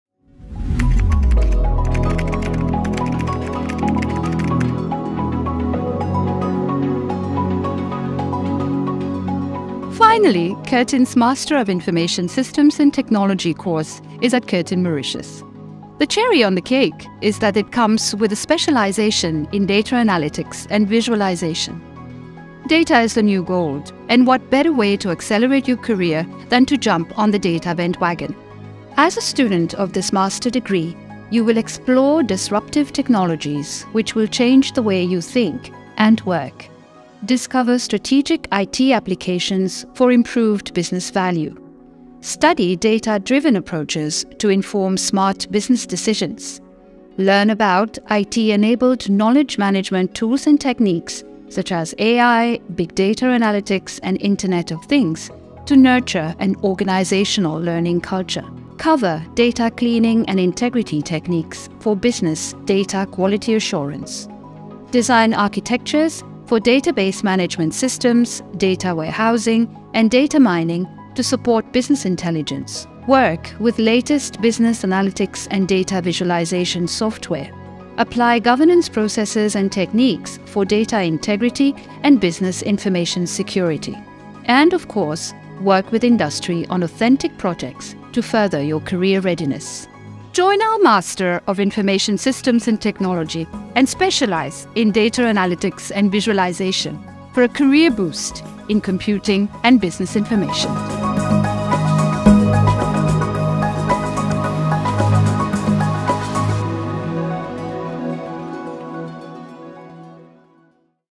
Master of Information Systems and Technology
The Curtin Master of Information Systems and Technology is designed to equip students with the skills and knowledge necessary to succeed in today's ever-evolving technological landscape. With a focus on practical/hands-on learning, students will be exposed to real-world scenarios that will prepare them to tackle complex IT challenges. This Postgraduate Degree will allow students to specialise in data and gain the expertise needed to excel in the field.